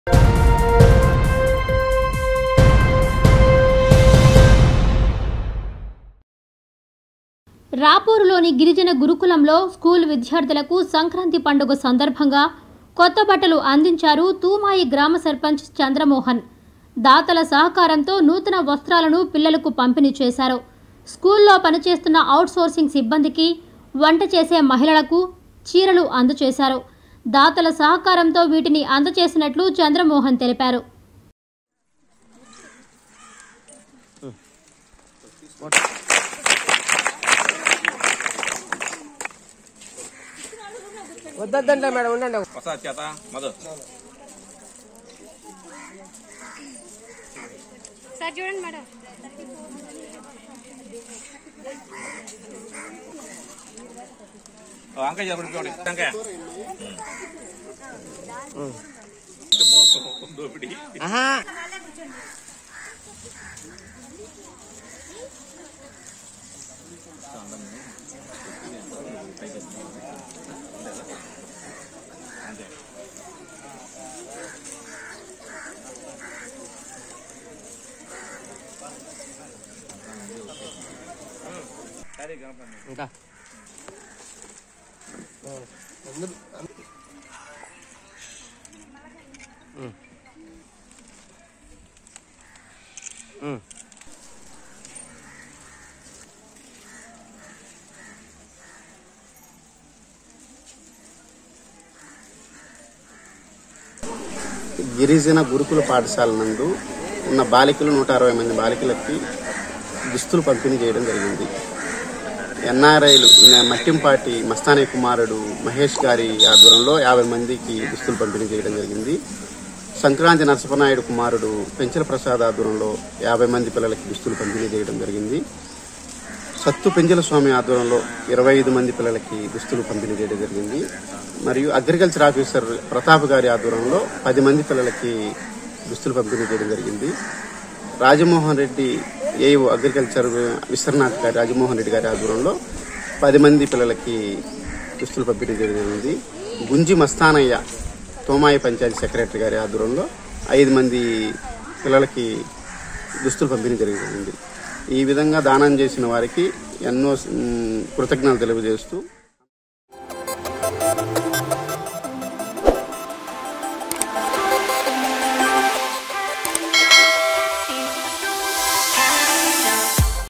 గురుకులంలో విద్యార్థులు.. కొత్త బట్టలు చూసి ఎలా సంబరపడుతున్నారో చూడండి.. - NDN News
రాపూరులోని గిరిజన గురుకులంలో స్కూల్ విద్యార్ధులకు సంక్రాంతి పండుగ సందర్భంగా కొత్త బట్టలు అందచేశారు. తుమాయి గ్రామ సర్పంచ్ చంద్రమోహన్.. దాతల సహకారంతో నూతన వస్త్రాలను పిల్లలకు పంపిణీ చేశారు. స్కూల్ లో పనిచేస్తోన్న ఔట్ సోర్సింగ్ సిబ్బందికి, వంటలు చేసే మహిళలకు చీరలు అందచేశారు. దాతల సహకారంతో వీటిని అందచేసినట్లు చంద్రమోహన్ తెలిపారు.